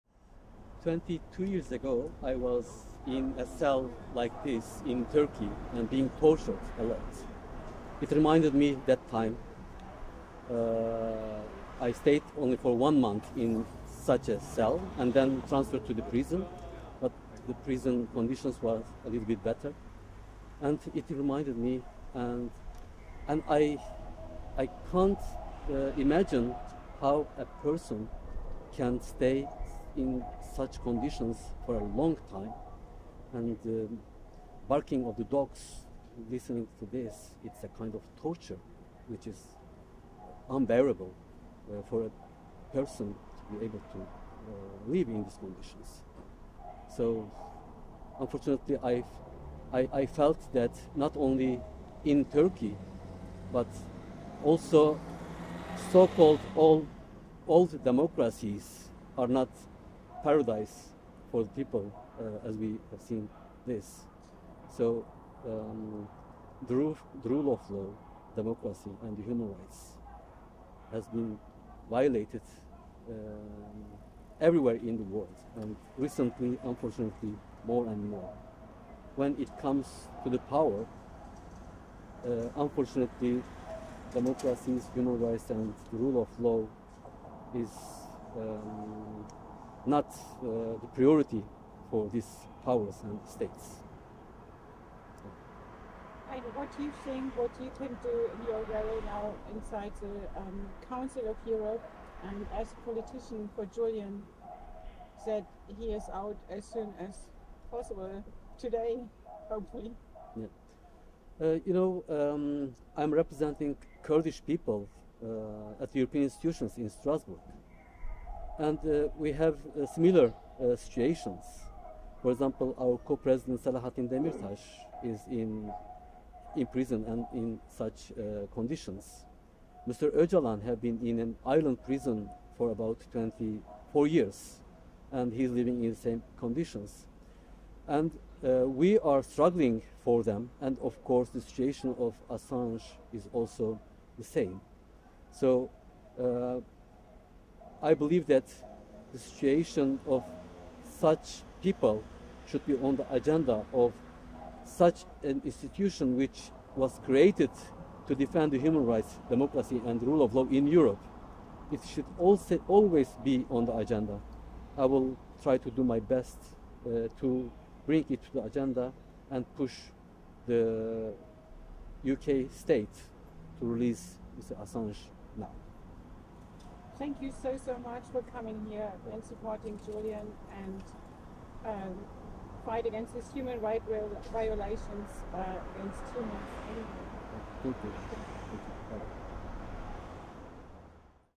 Assange: Kurdish HDP at Belmarsh Live (Oct. 12 2023)
OCTOBER 2023, STRASBOURG: Fayik Yagizay, HDP (Peoples' Democratic Party of Turkey) representative in Europe, visits Belmarsh Live, a mobile recreation of Julian Assange's 2 x 3–meter Belmarsh Prison cell in London. It is Oct. 12, the last of three days during which the installation is parked opposite the Council of Europe on Avenue de l'Europe. Yagizay, a Kurd, says being inside the cell — albeit for a few minutes — reminds him of his own imprisonment and torture 22 years earlier in Turkey.